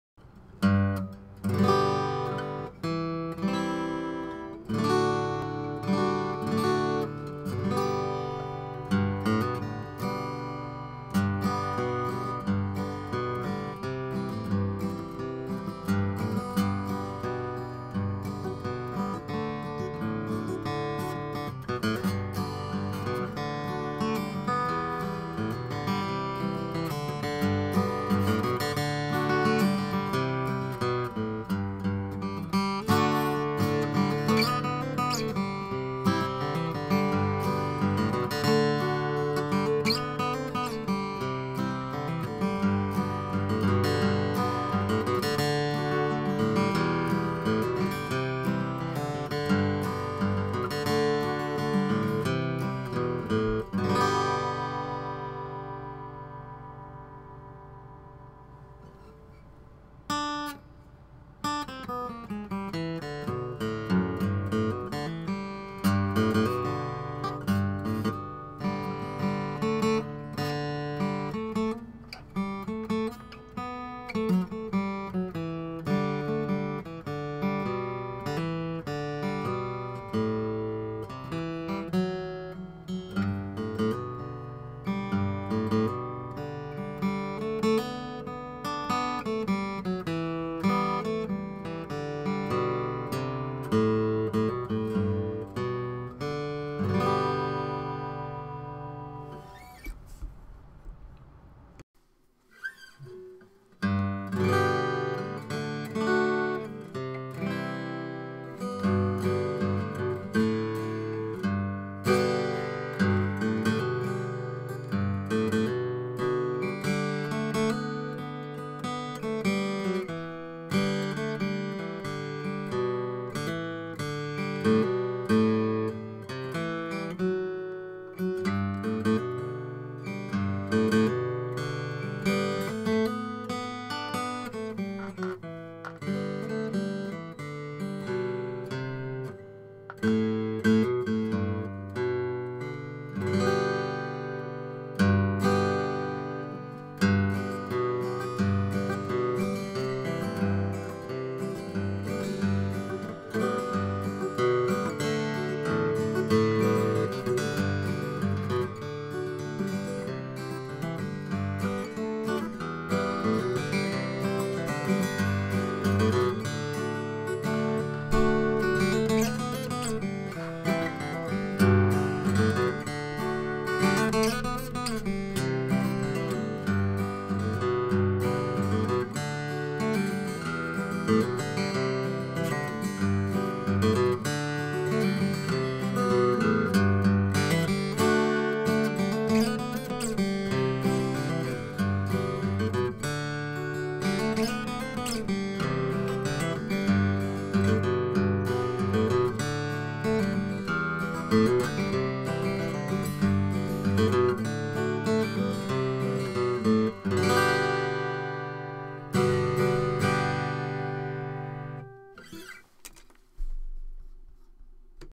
Carlo Robelli J-220-VS (J45 Clone) Brace shave before and after
I took the #3 and #4 braces on the back of this inexpensive J45 clone down to 1/2". (Back braces come stock at 1" tall, 5/16" wide, tapered to a point.) Before has the pickguard on, and after has the pickguard off. One take, sore fingers, very tired. Not my best playing.

To my ear, the bass is more pronounced and "woofy" in the after section, what I wanted to hear from a slope shouldered dreadnaught. Same strings, etc. After I recorded the after I swapped for a bone nut and saddle and bone pins. More clarity and punch now, too.